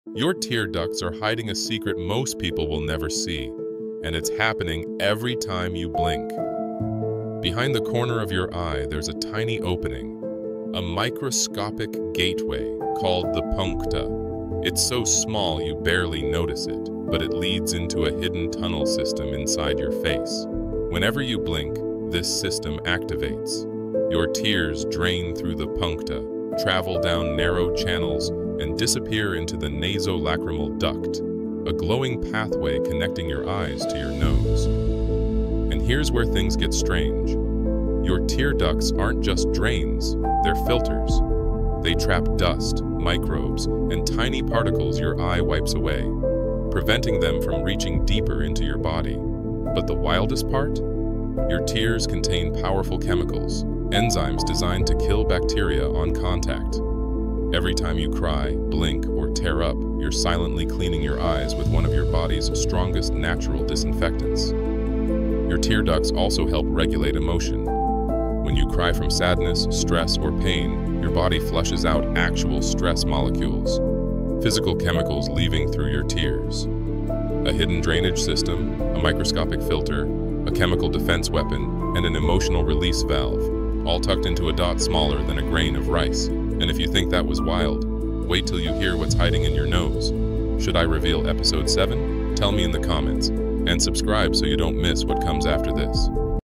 The Secrets Hiding in Your Tear Ducts — What Happens Every Time You Blink
sixty seconds to see the hidden system behind your eyes — the microscopic tunnels that drain your tears, filter out microbes, and release emotional chemicals every time you blink.
Your tear ducts aren’t just drains; they’re biological filters, disinfectant channels, and emotional release valves that protect your eyes and your mind.
This short reveals the secret anatomy of your puncta, the glowing pathways inside your face, and the powerful enzymes your tears carry.
Watch till the end… the final detail will change how you think about crying forever.